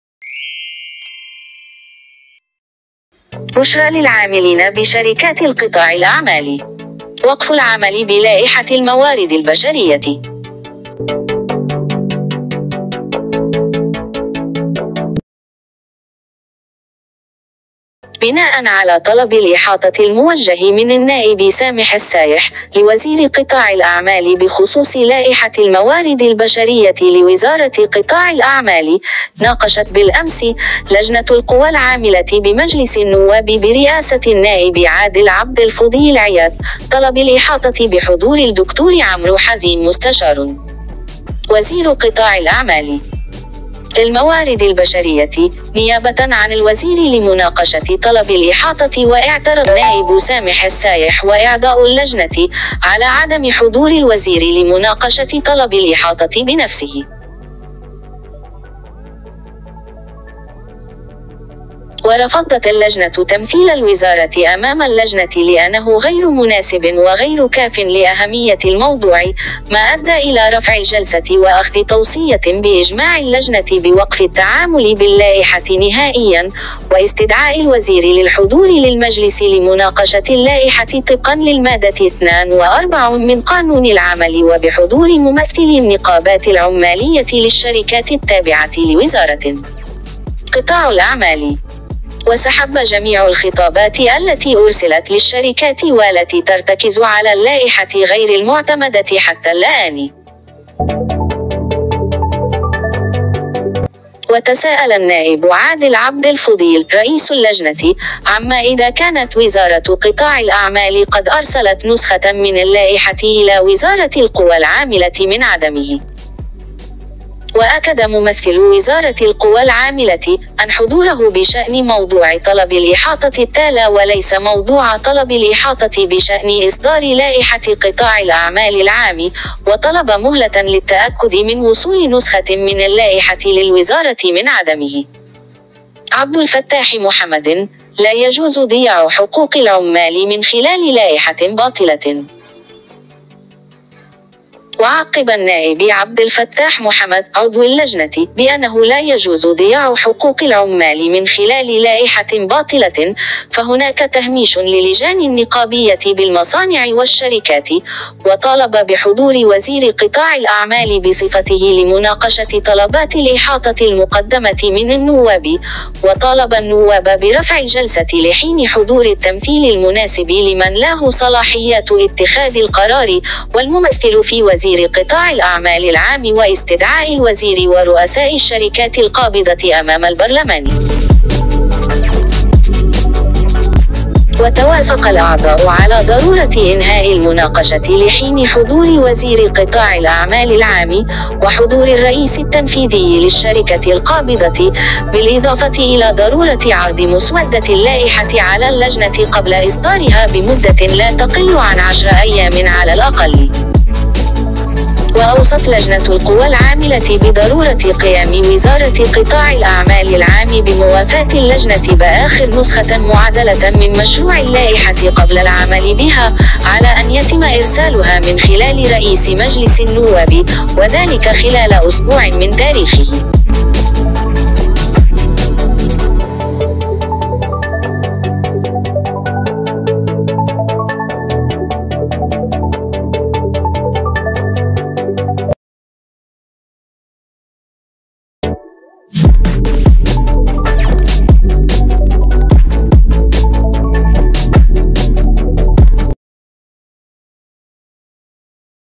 بشرى للعاملين بشركات القطاع الأعمال.. وقف العمل بلائحة الموارد البشرية
بناء على طلب الإحاطة الموجه من النائب سامح السايح - لوزير قطاع الأعمال بخصوص لائحة الموارد البشرية لوزارة قطاع الأعمال، ناقشت بالأمس، لجنة القوى العاملة بمجلس النواب برئاسة النائب عادل عبد الفضيل عياد، طلب الإحاطة بحضور الدكتور عمرو حزين مستشار وزير قطاع الأعمال للموارد البشرية - نيابة عن الوزير لمناقشة طلب الإحاطة واعترض النائب سامح السايح واعضاء اللجنة، على عدم حضور الوزير لمناقشة طلب الإحاطة بنفسه.